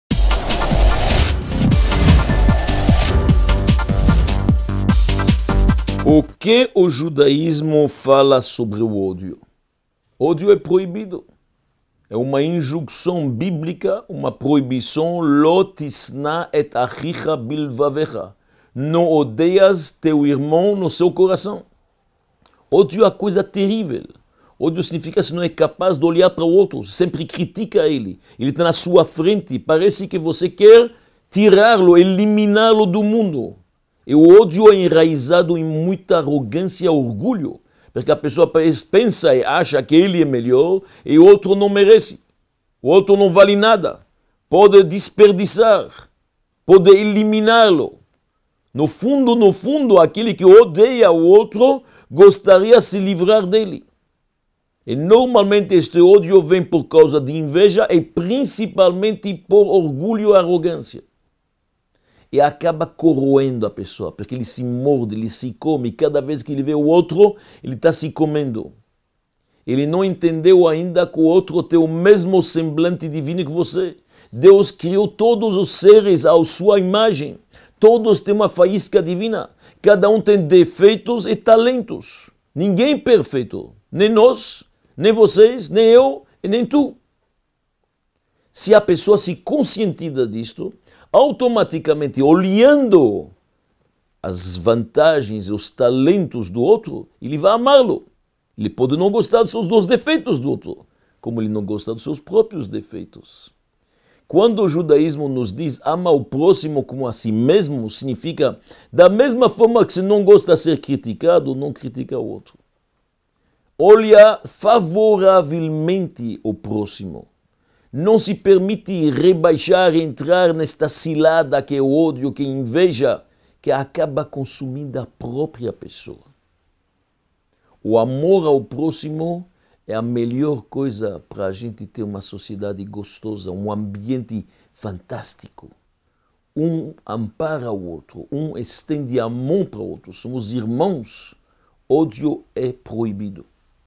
Rabino David Weitman
O que o judaísmo fala sobre o ódio?